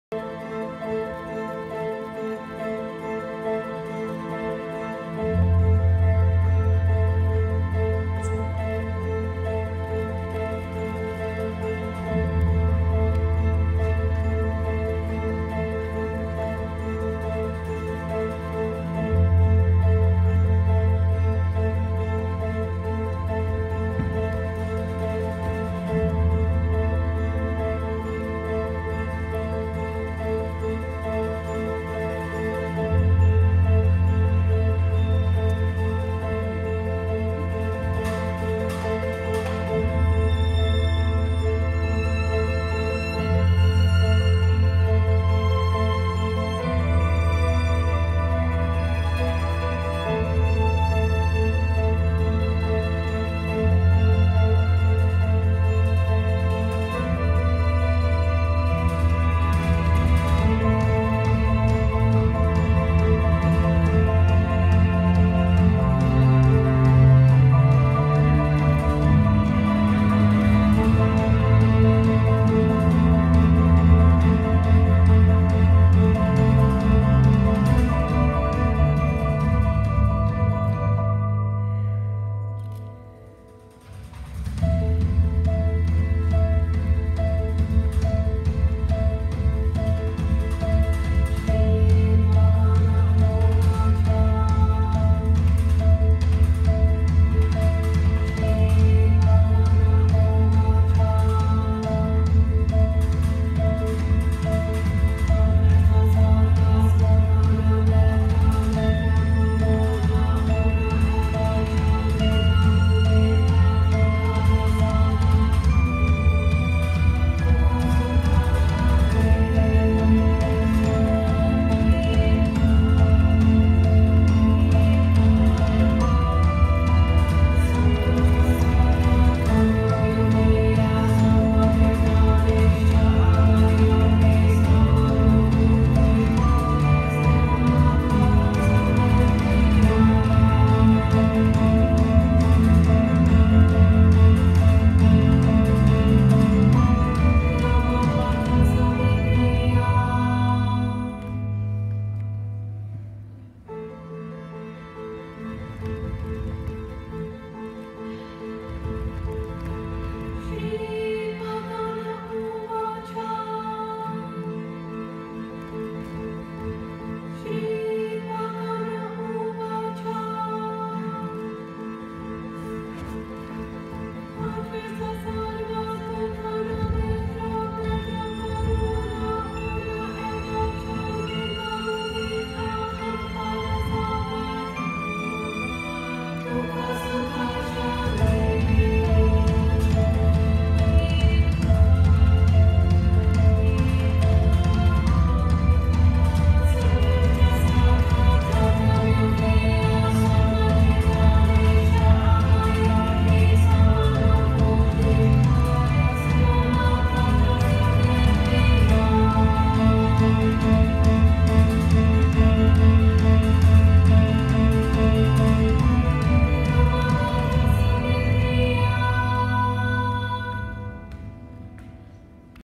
A3 Dance Company “Visions of the Future” 4.24.2019
The Academy for Arts and Academics presents the Spring Dance Concert 2019
Rehearsal run through
The Dougherty Dance Theater
University of Oregon
Choreography by Erinn Ernst
Costumes: Ariana Schwartz and Georgia Bryant